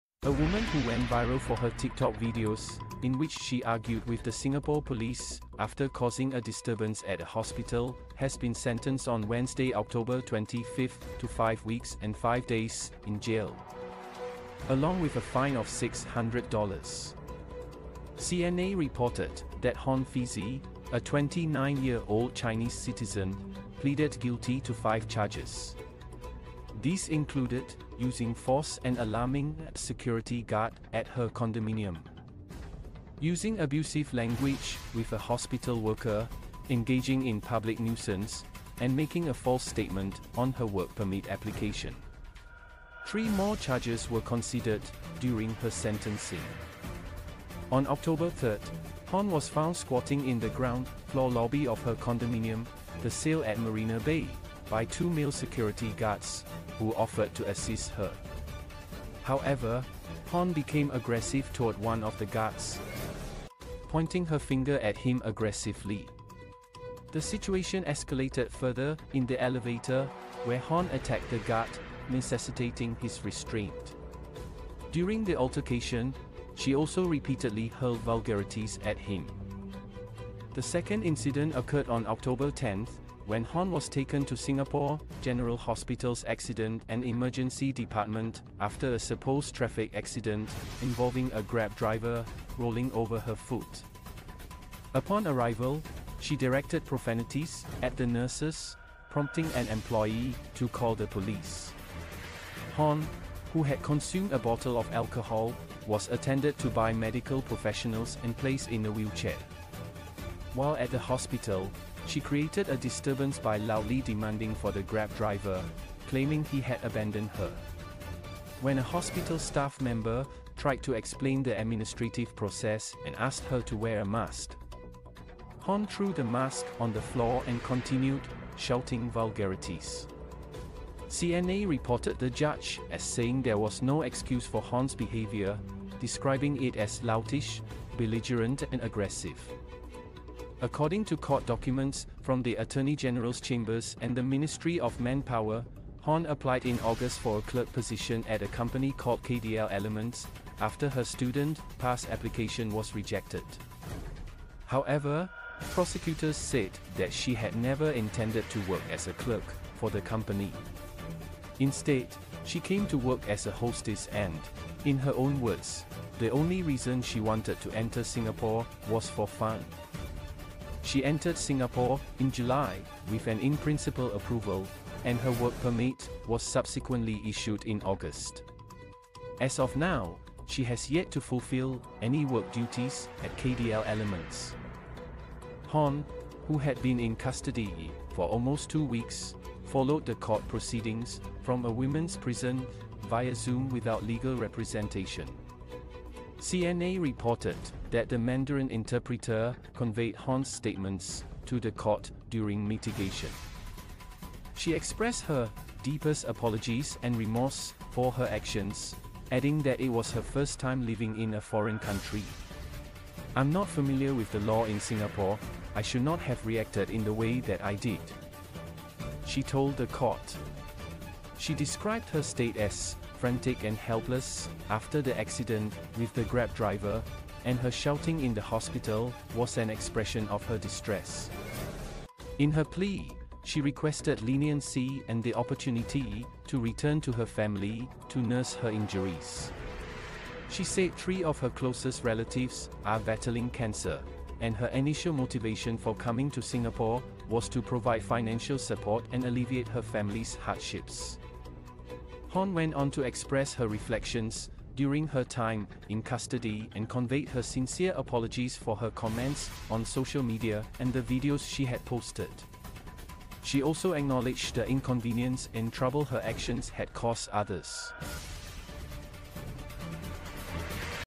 Woman in Viral Video Faces Jail Time and Fines After Hospital Argument
- A woman in Singapore who went viral for a video of her arguing with hospital staff has been sentenced to jail and fined.
- The woman was charged with multiple offenses, including harassment and causing a public nuisance.
- In the video, she can be seen shouting and using vulgar language towards hospital staff who were trying to help her.
- The woman's behavior was deemed unacceptable and a threat to public order.
- She was sentenced to 10 days in jail and fined SGD 2,000 (approximately USD 1,500).

💗📌 Singapore